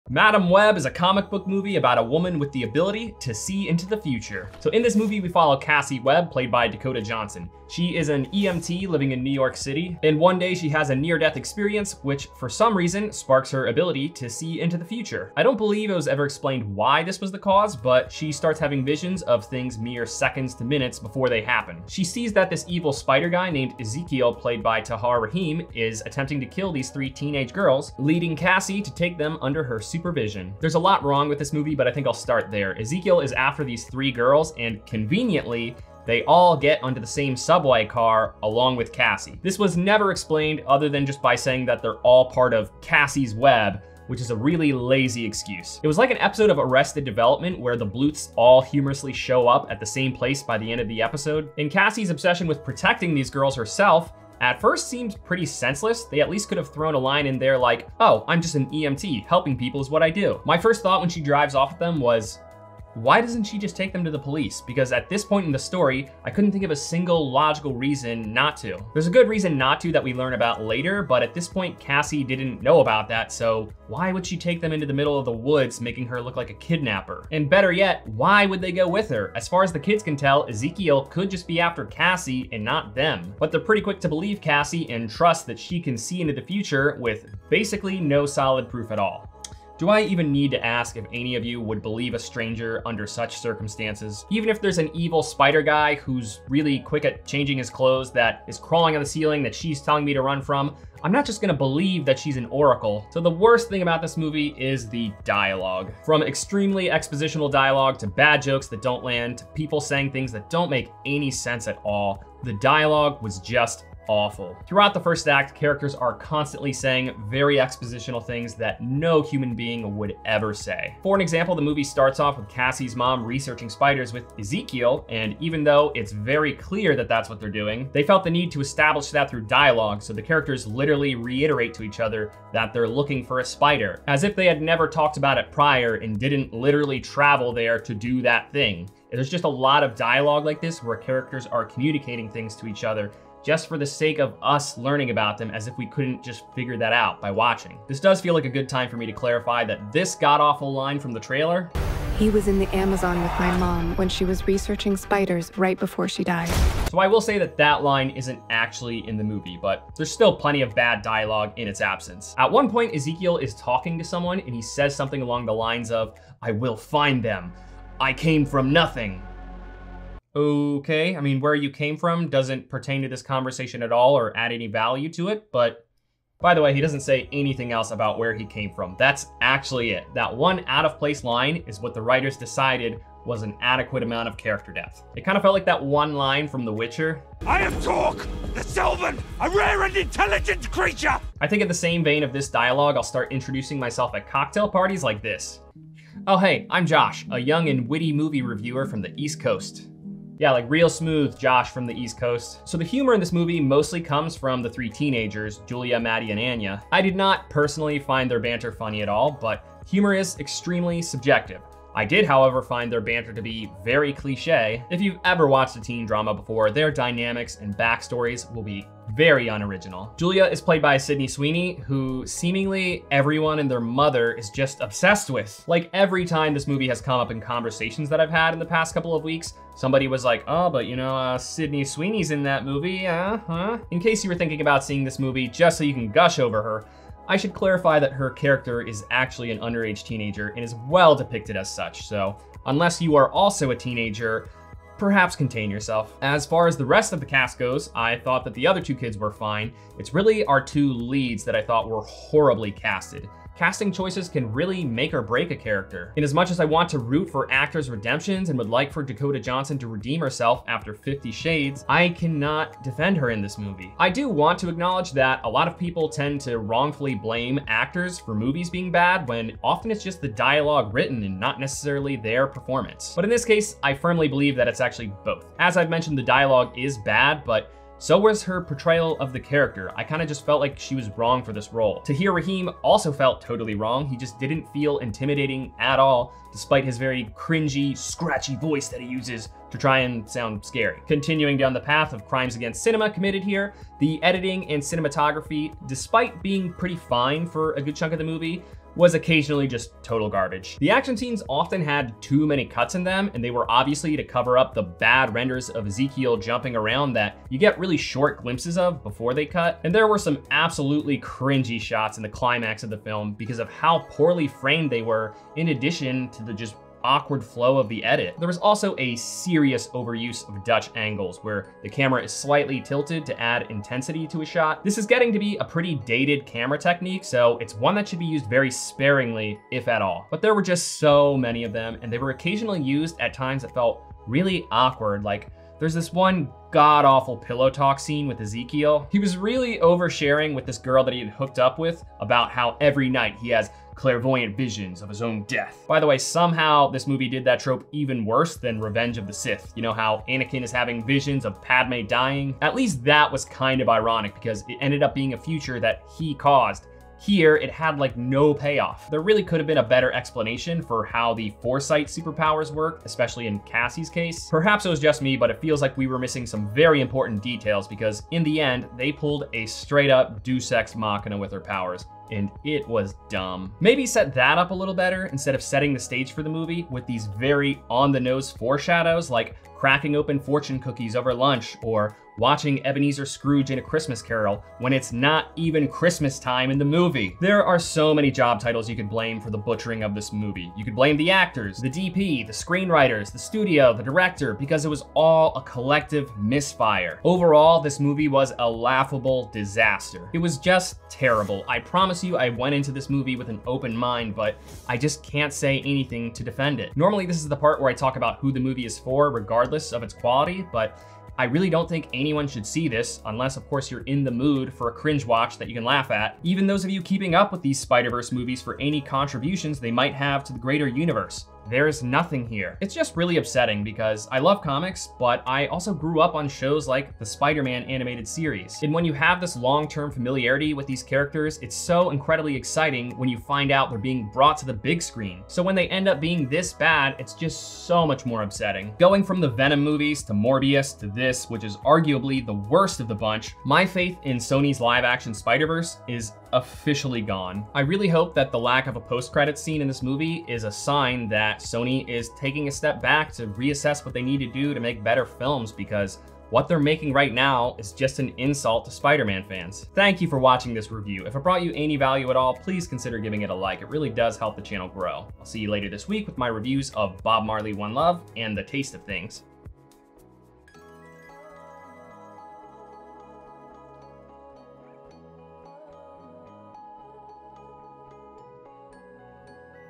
MADAME WEB Review - This is NOT the Spiderverse We Deserve
Sony's newest live action spiderverse movie continues an unfortunate trend for the studio.